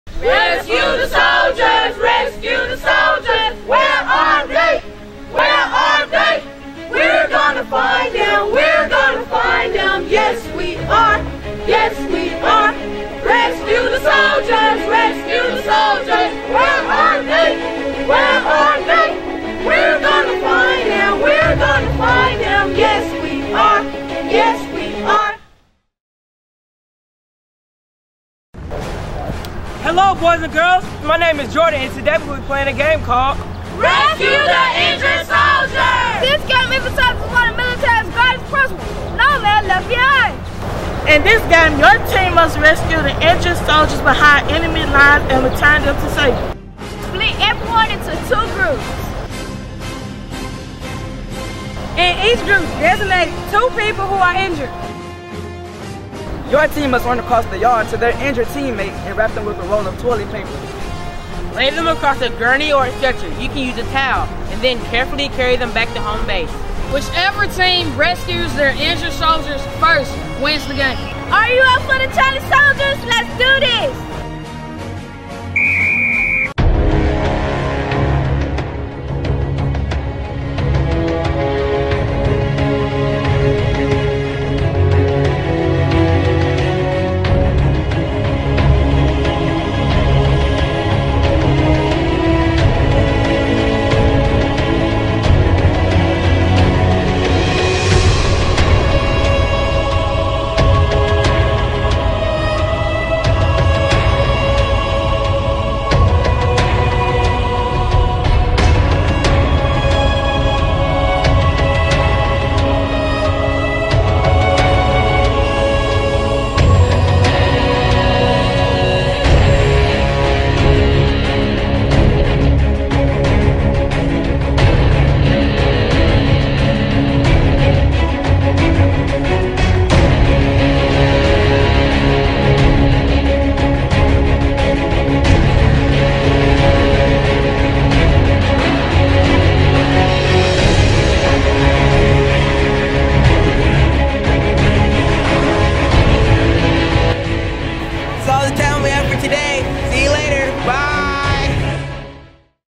Rescue the Soldier - JTS Game for Kids
This is a game emphasizing one of the military’s greatest principles: No Man Left Behind.
In this game your team must rescue injured soldiers behind enemy lines and return them to safety.
Split everyone into two groups
In each group designate two people who are injured.
Your teams have to run across the yard to their injured teammates and wrap them up with a whole roll of toilet paper, lay them on the gurney or stretcher (beach towel) and carefully carry them (one kid at each corner) to the home base.
They then  run back to get the other injured soldier, wrap & carry them back to be the first team to win the race.
Are you up to the challenge soldiers? Let’s do this!